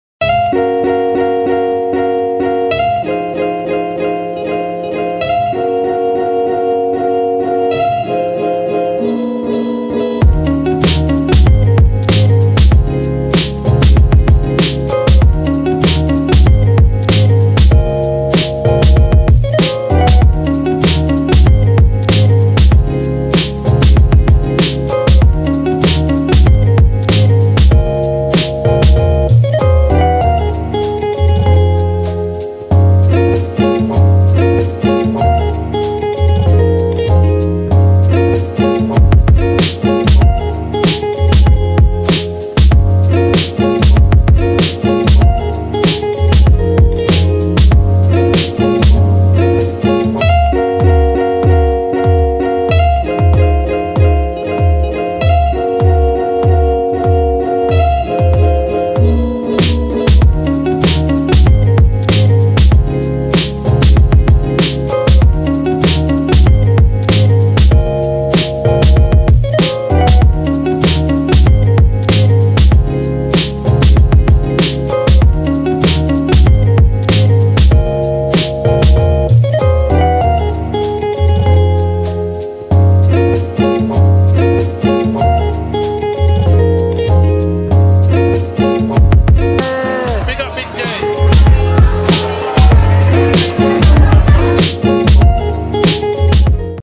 Streats checks out Big J's Street Food Clash!
We went down to Croydon to go and check out Big J's Street Food Clash. The drinks were flowing, the music was pumping and the food was unreal.

Big thanks to Big J for the invite!